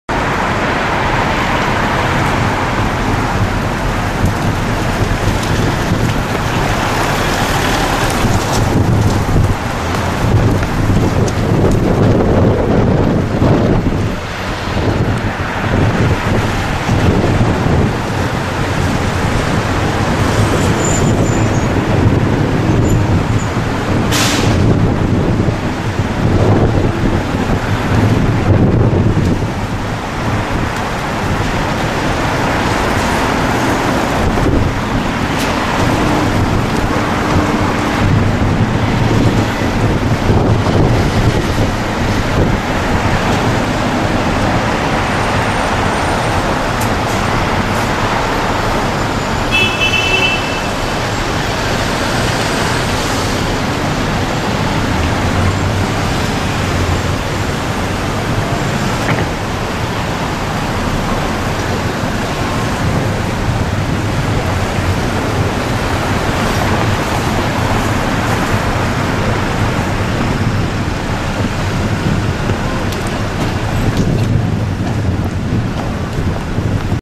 서울에 첫눈 내려
서울 지역에 18일 오후에 첫 눈이 내렸다.